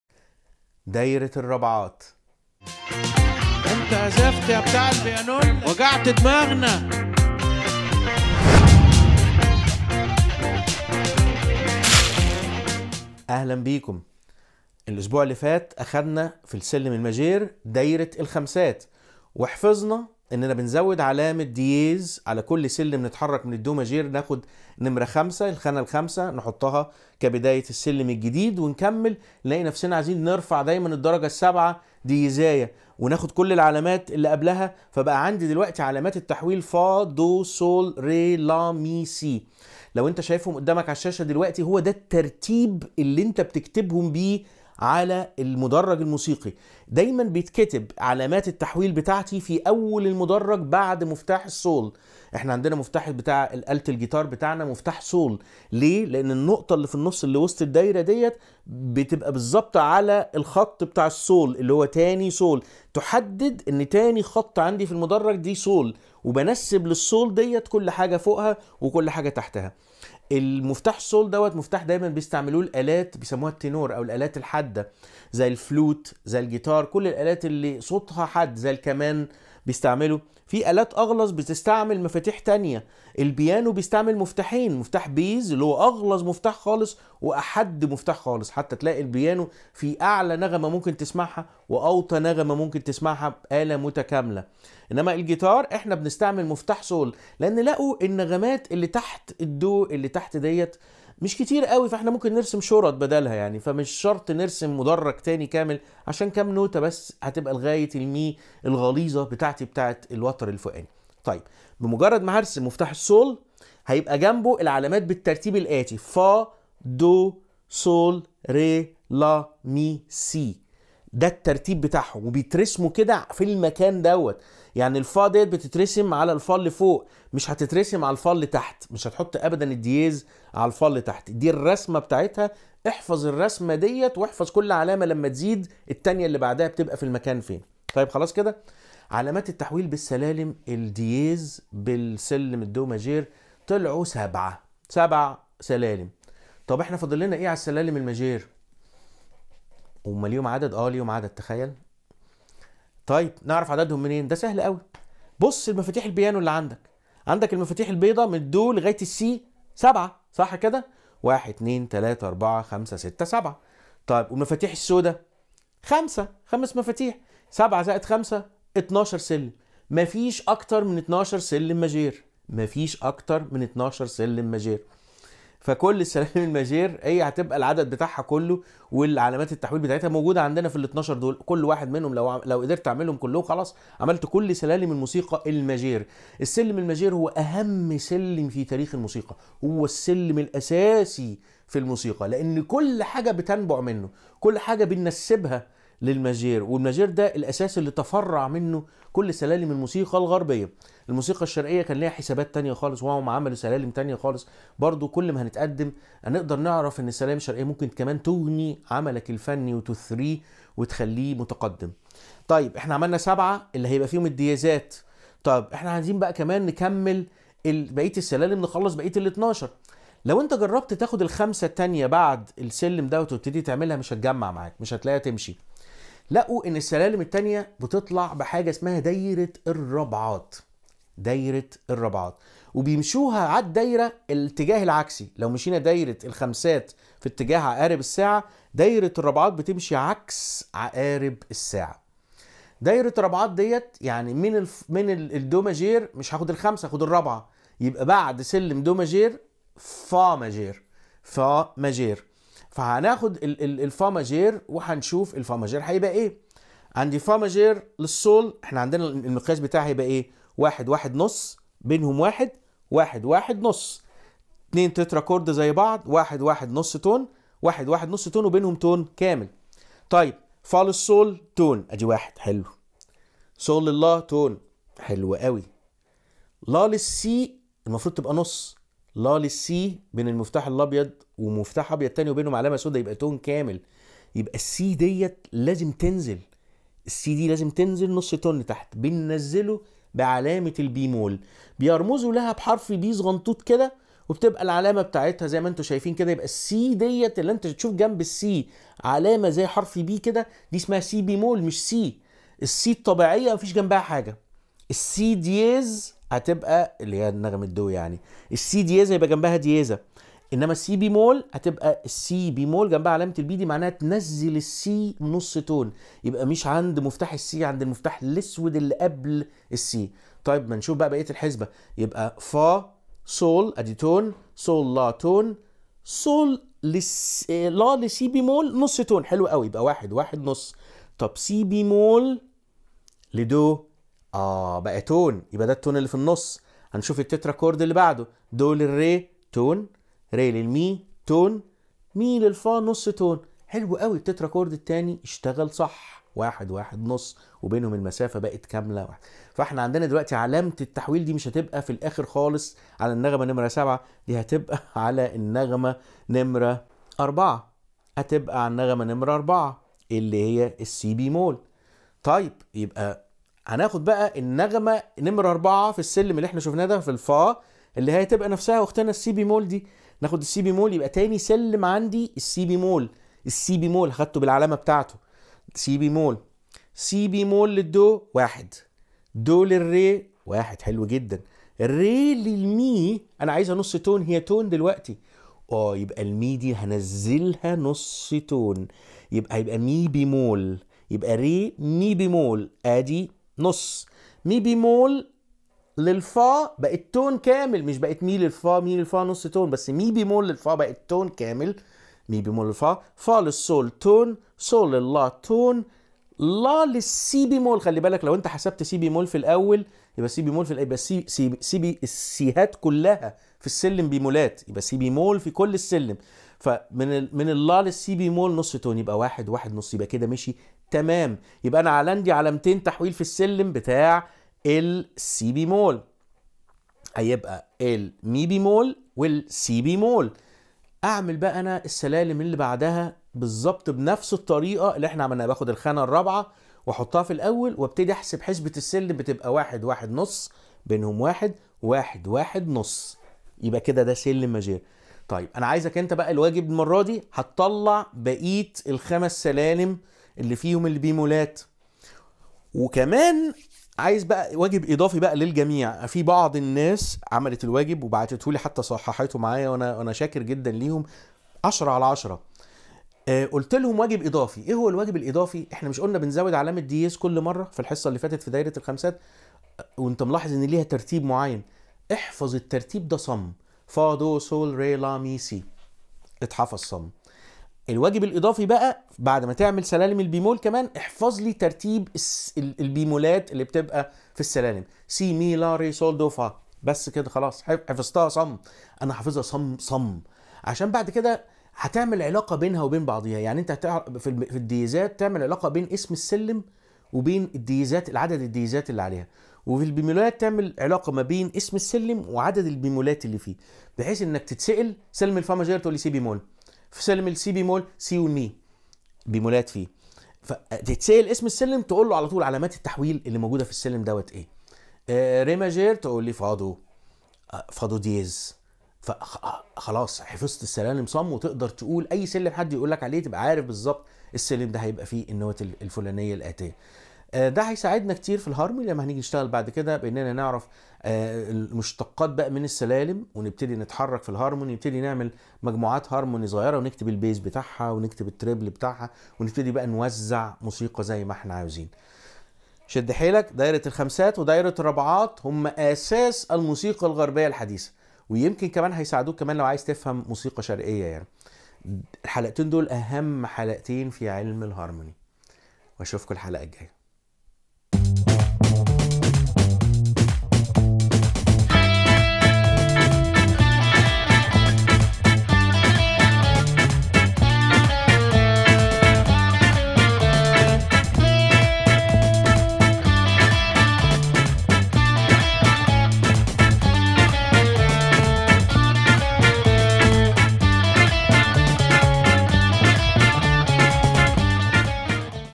تعليم جيتار - هارموني - دائرة الرابعات -- Harmony - cycle of fourths
درس جديد من دروس الهارموني يوضح استخراج باقي السلالم الماجير عن طريق اضافة علامات البيمول لتكتمل كل سلالم الماجير الآن